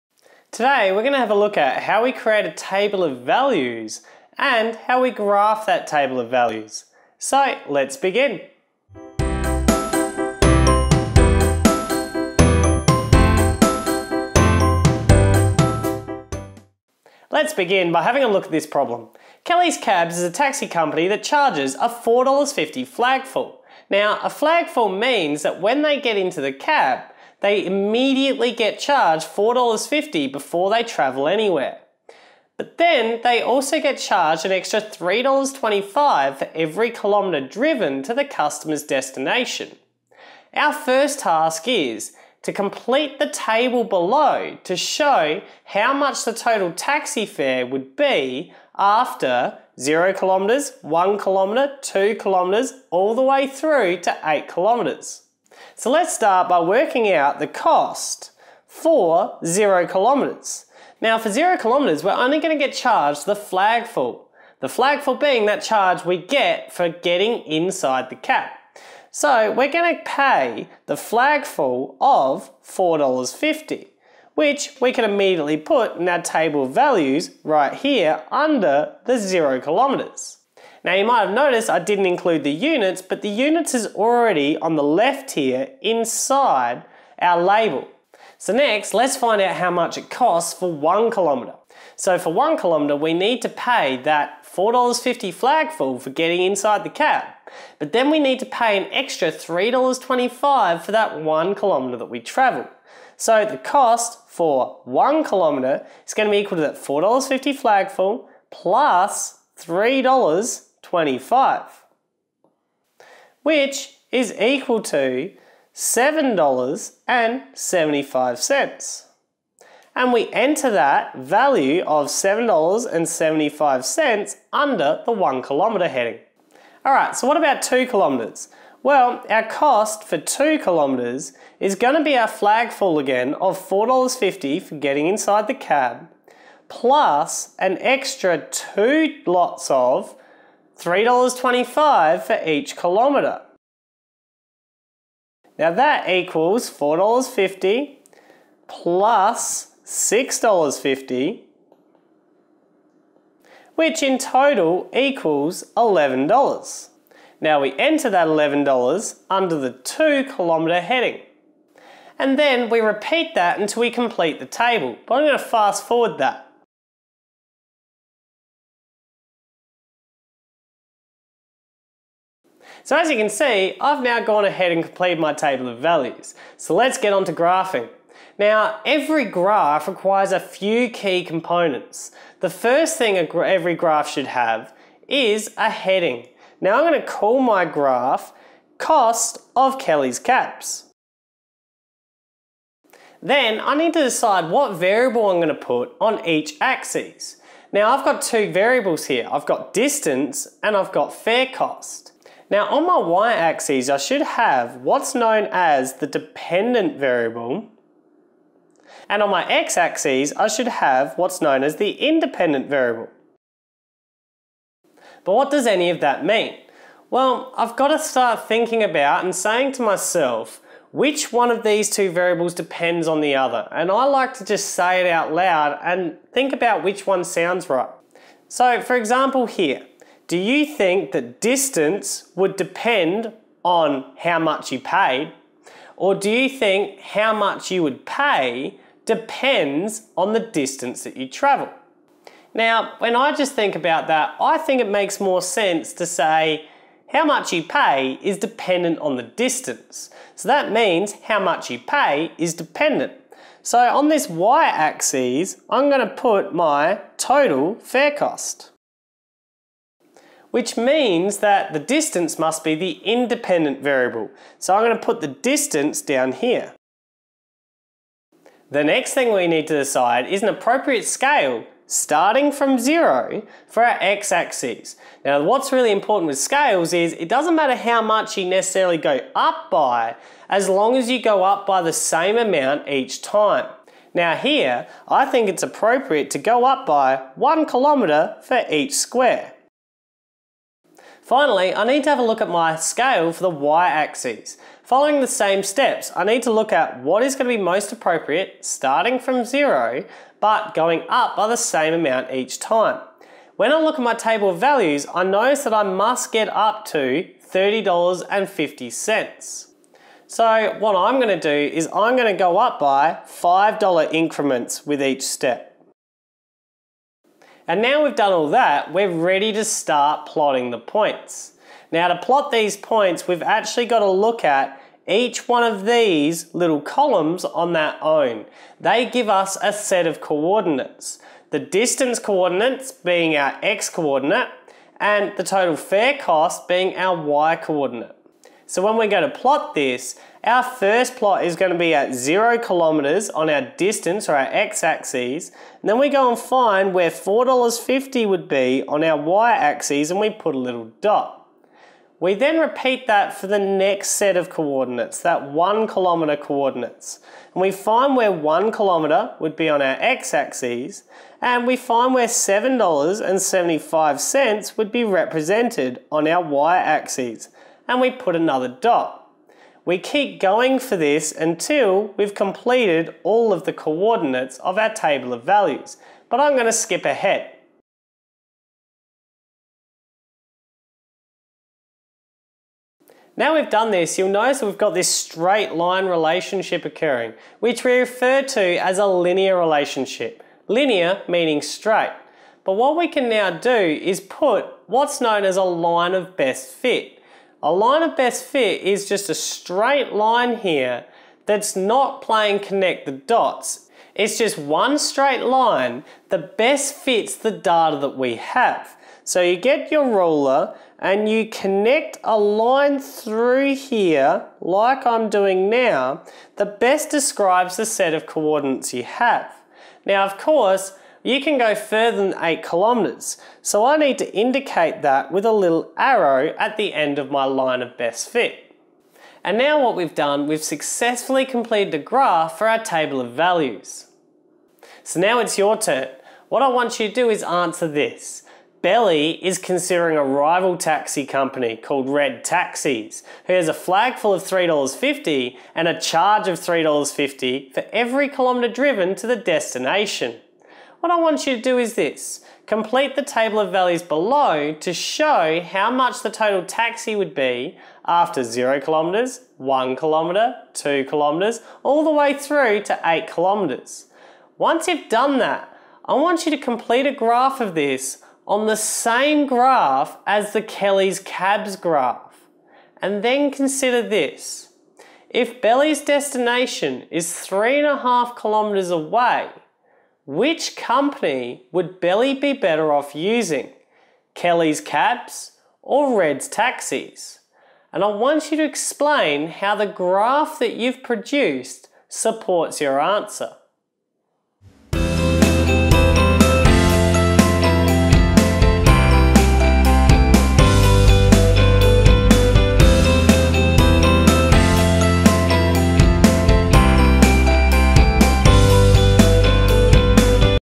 Linear Relationships: Table Values and Graphing
In this video I demonstrate how to produce a table of values and then graph that table of values for a problem.